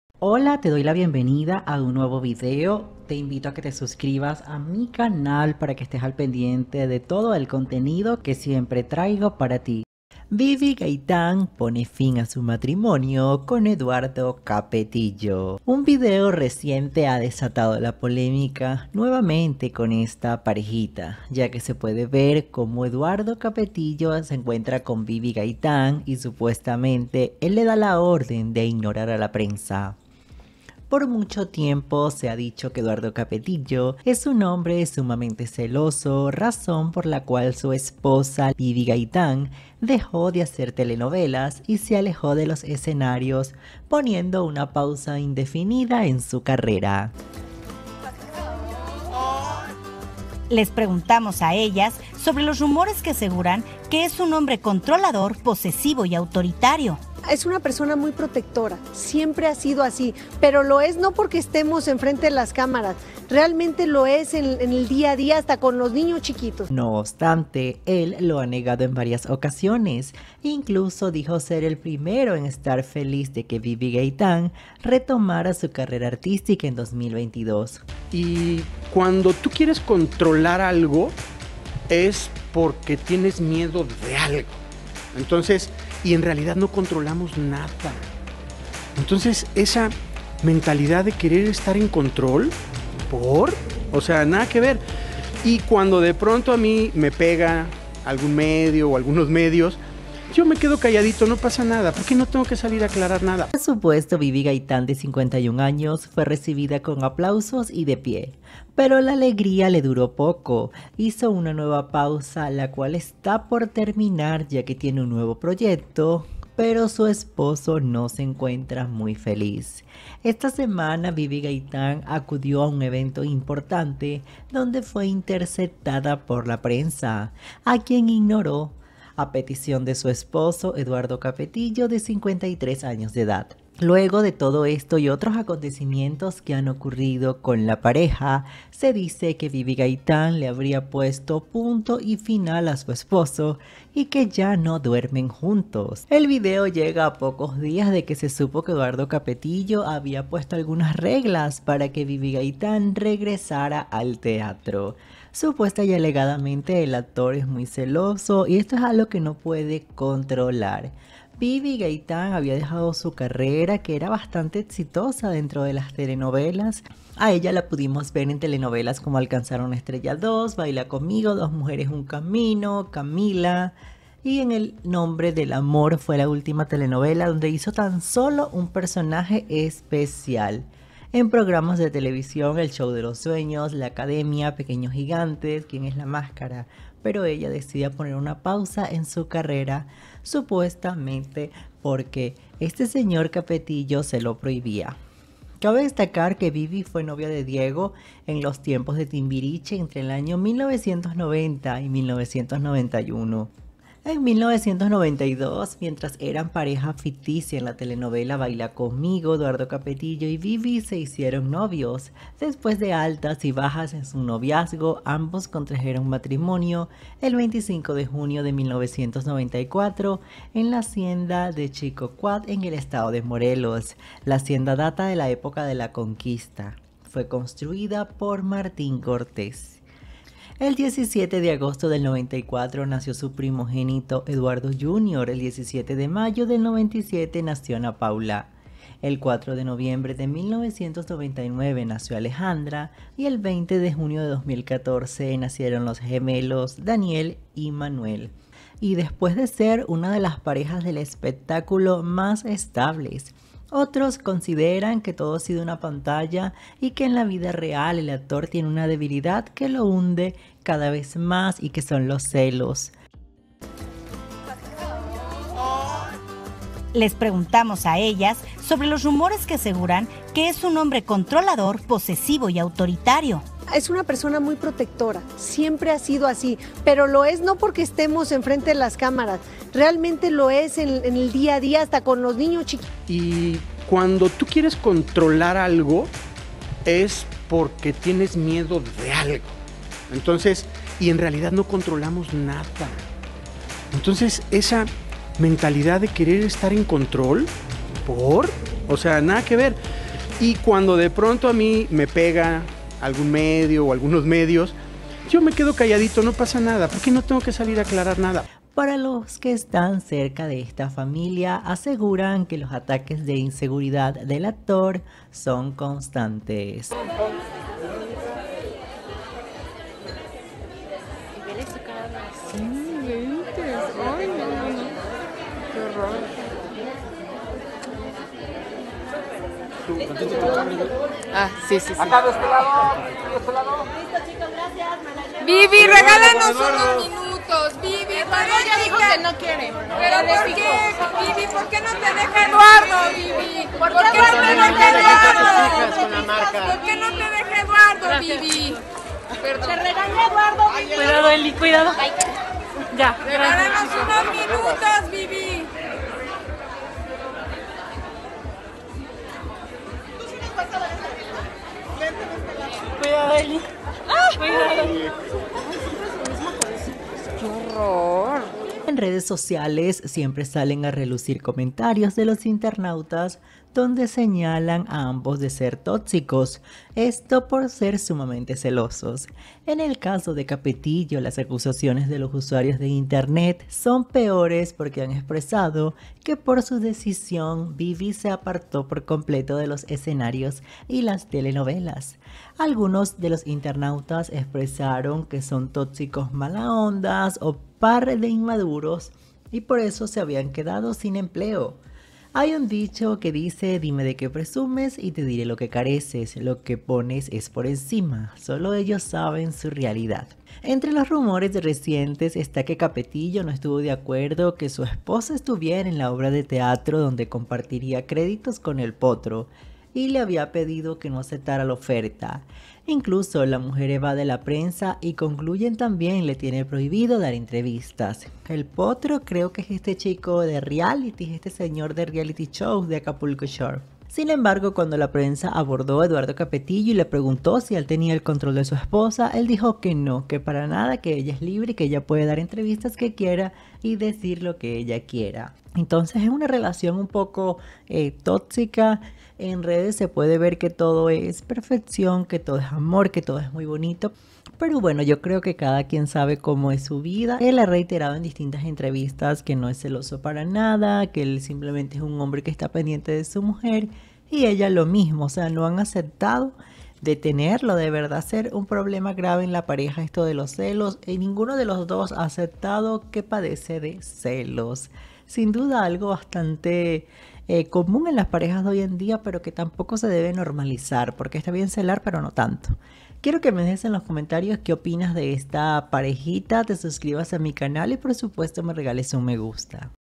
Biby Gaytán confirma FIN DE SU MATRIMONIO con Eduardo Capetillo
El 25 de junio de 1994, contrajeron matrimonio los actores Bibi Gaytán y Eduardo Capetillo. Su matrimonio, con cinco hijos, es considerado uno de los más estables del mundo del espectáculo mexicano; sin embargo, desde siempre se ha mencionado que el actor controla en exceso a su esposa y es quién decide en qué proyectos y con quiénes puede hablar; situación que, si bien ha sido desmentida en repetidas ocasiones por la exintegrante de Timbiriche, continúa siendo un tema recurrente.

 Ante ello, recientemente se ha filtrado información de que la actriz ha tomado la decisión de separarse .